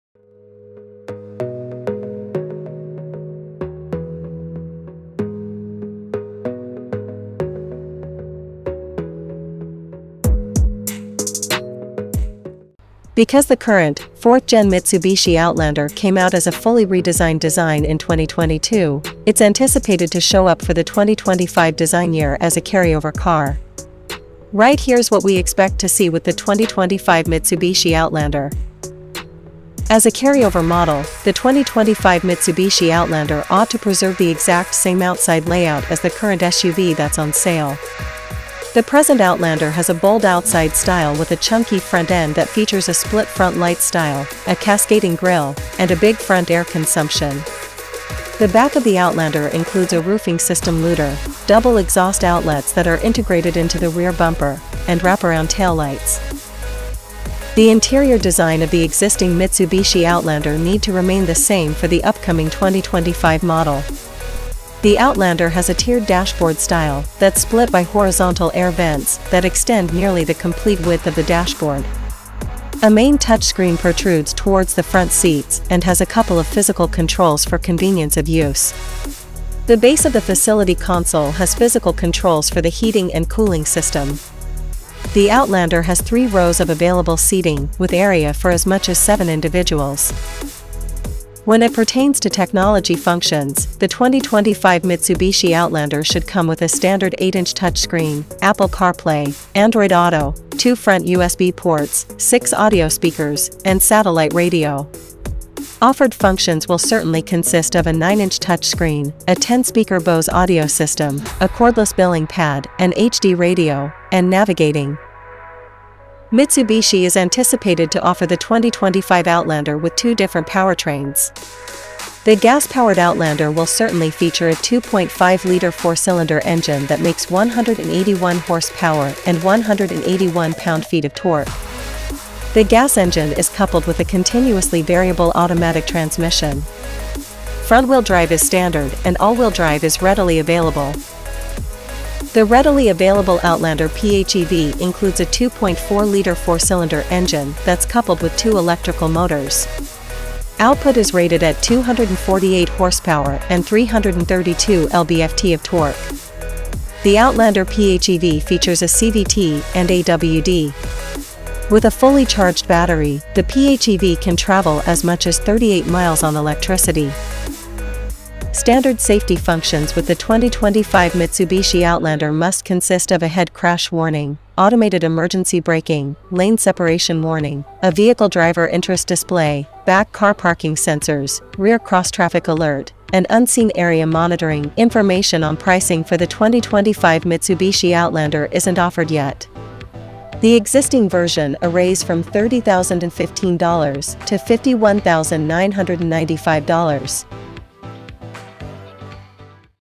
Amazing Powerful SUV - New 2025 Mitsubishi Outlander Redesign
2025 Mitsubishi Outlander SUV | The 2023 Mitsubishi Outlander PHEV marked a significant redesign, boasting a more powerful engine, Super All-Wheel Control (S-AWC) as standard, a longer all-electric range, a new suite of technology, an extra row of seats, and many more upgrades. As we look forward to the 2025 model, we can expect further enhancements and refinements to this impressive vehicle.

Inside the cabin, the 2025 Mitsubishi Outlander will likely offer a modern and comfortable environment, building on the improvements made in the 2023 model. With high-quality materials, an intuitive infotainment system, and a range of available luxuries, such as quilted leather seats and a panoramic sunroof, the Outlander will provide a premium driving experience.